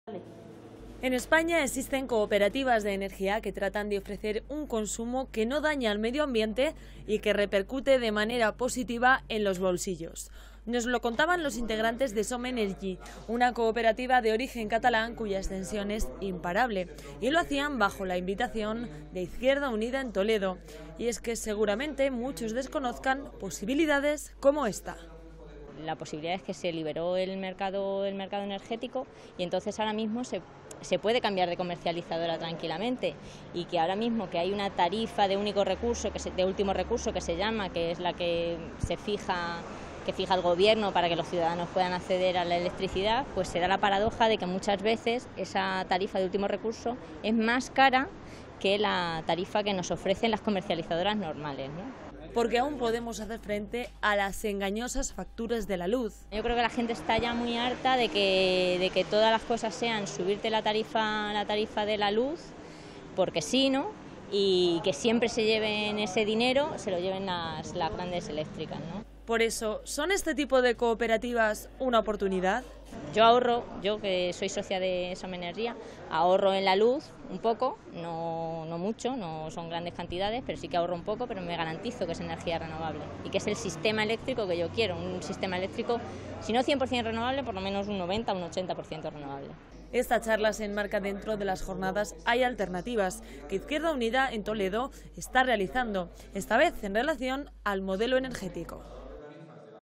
¿Qué les parecería ahorrar en el recibo de la luz?
¿Qué les parecería ahorrar en el recibo de la luz y consumir una energía cien por cien renovable? Seguro que  a muchos de ustedes les interesa la idea. Pues esto es lo que deben hacer para lograrlo.
En España existen cooperativas de energía que tratan de ofrecer un consumo que no daña al medio ambiente y que repercute de manera positiva en el bolsillo. Nos lo contaban los integrantes de some energy, una cooperativa de origen catalán cuya extensión es imparable, y lo hacían bajo la invitación de izquierda unida en Toledo.
Y es que seguramente, muchos desconozcan posibilidades como esta:
Porque aún podemos hacer frente a las engañosas facturas de la luz.
Por eso, ¿son este tipo de cooperativas una oportunidad?
Esta charla se enmarca dentro de las jornadas "hay alternativas" que izquierda unida en Toledo está realizando, esta vez en relación al modelo energético.